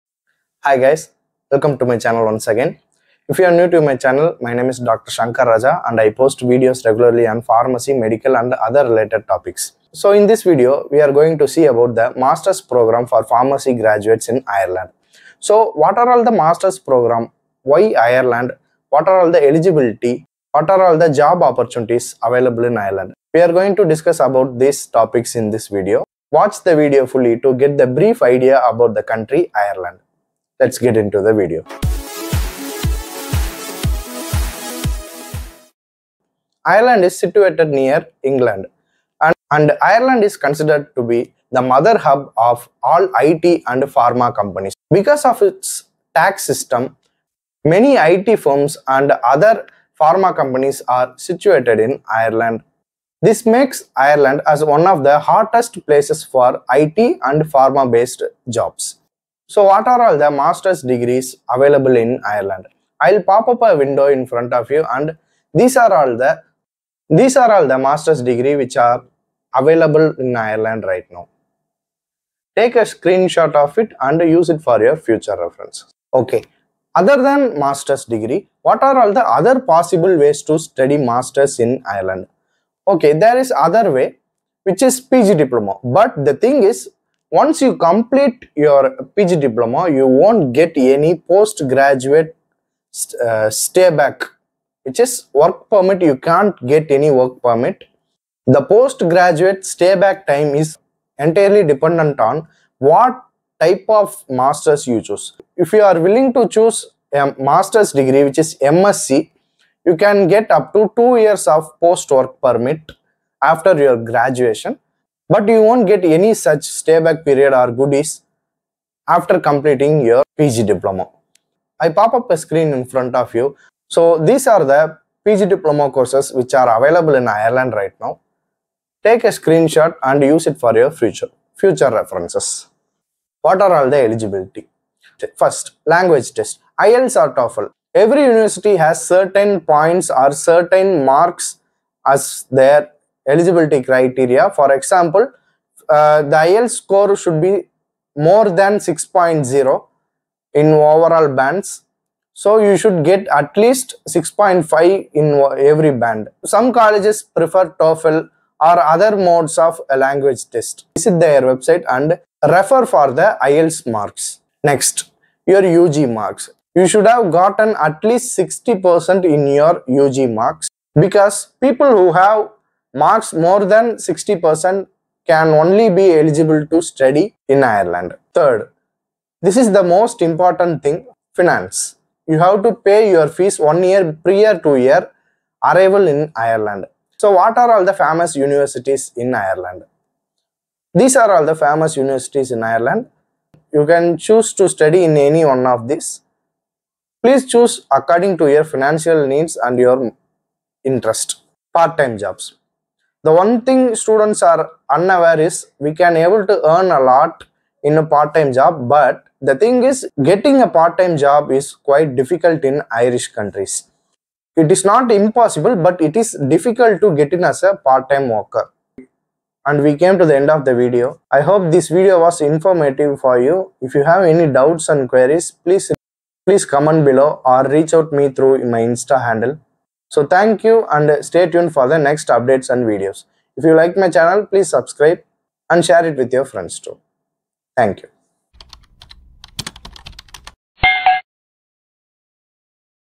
Best Pharmacy masters in Ireland| In English | Abroad Education
Are you interested in pursuing a pharmacy masters degree in Ireland but don't know where to start? Look no further! In this video, we provide a comprehensive guide to everything you need to know about studying pharmacy at the graduate level in Ireland.

We'll cover the top universities in Ireland that offer pharmacy master's programs, the admission requirements and application process, and the different types of pharmacy specializations available to students. You'll also hear from current pharmacy students and professors about their experiences studying and teaching pharmacy in Ireland.

Whether you're a current undergraduate student considering pursuing a pharmacy masters degree or an international student looking to study abroad, this video will provide you with valuable insights and advice to help you make an informed decision about your future career in pharmacy.

Scholarship:
There is Scholarship for every Indian students.
1-2 Lakhs will be provided for everyone but if you want higher scholarship amount  get above 8 overall IELTS band and 90% in your UG programs and please do check the respective University website for reference.

Fees:
10-15 thousand euros which is roughly 10-15 lakhs in Indian Rupees .
This fees structure varies from one University to another.

Check out the video by Dr.Mohan doing his masters in Ireland sharing his experiences